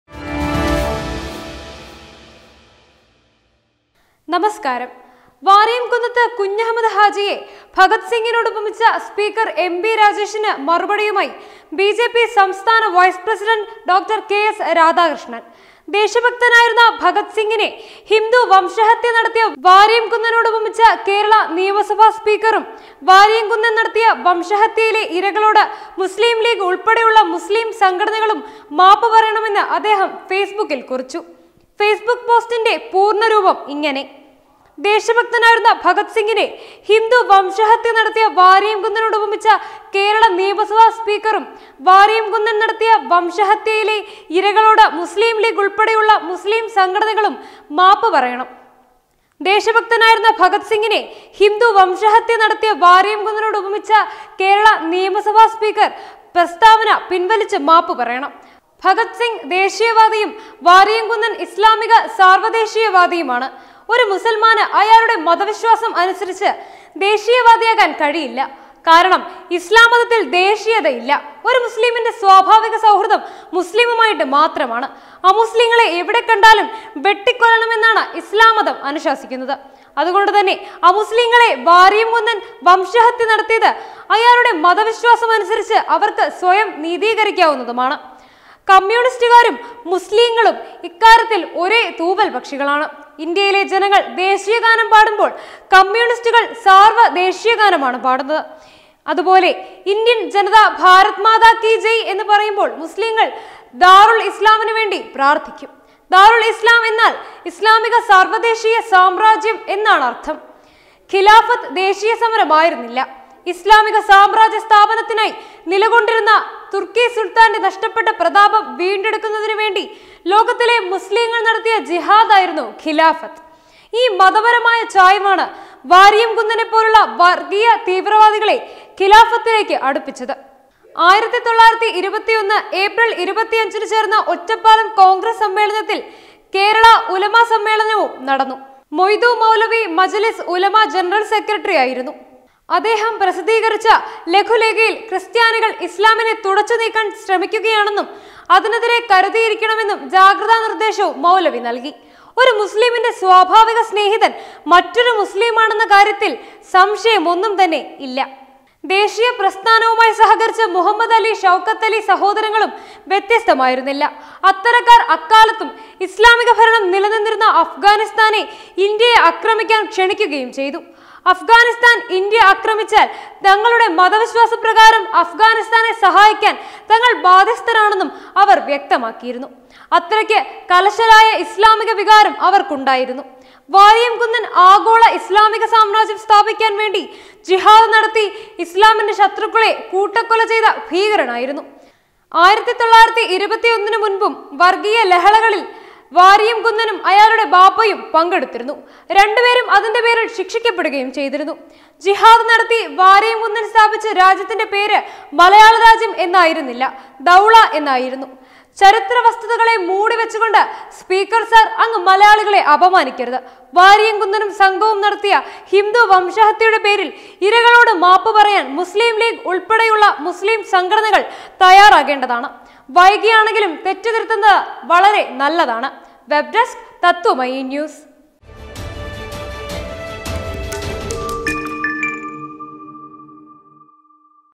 എം ബി രാജേഷിനെതിരെ ആഞ്ഞടിച്ച് കെ എസ് രാധാകൃഷ്ണൻ | K. S. RADHAKRISHNAN
മതഭ്രാന്തനെങ്ങനെ ഭഗത്‌സിംഗിനു തുല്യനാവും?
എം ബി രാജേഷിനെതിരെ ആഞ്ഞടിച്ച് കെ എസ് രാധാകൃഷ്ണൻ